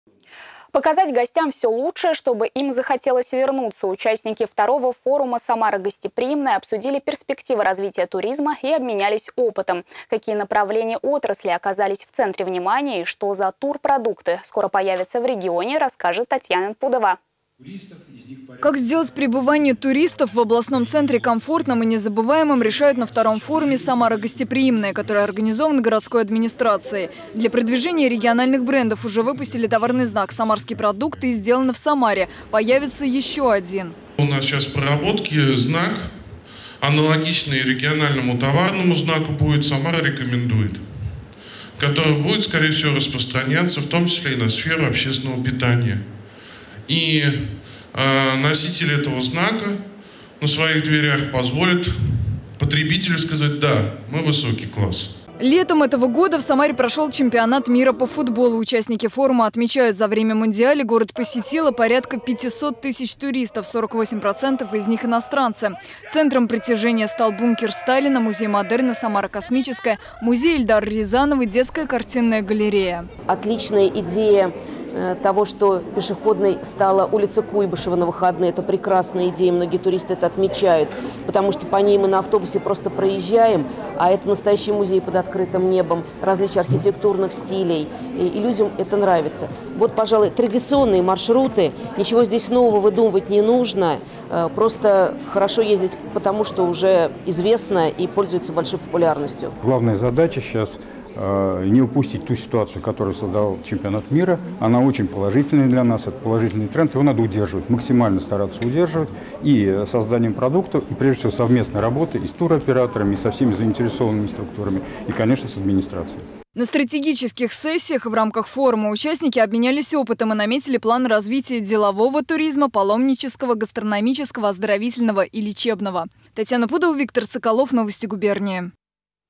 В областном центре проходит форум "Самара гостеприимная"
Участники мероприятия обсуждают вопросы о том, как сохранить туристический поток после мундиаля, сделать пребывание гостей комфортным. За время чемпионата мира по футболу Самару посетило порядка 500 тысяч туристов. 48 % из них - иностранцы. Центром притяжения стала улица Куйбышева, Бункер Сталина, Музей модерна, "Самара Космическая" и музей Эльдара Рязанова. В планах вывести на новый уровень деловой туризм, оздоровительный и лечебный, паломнический и гастрономический.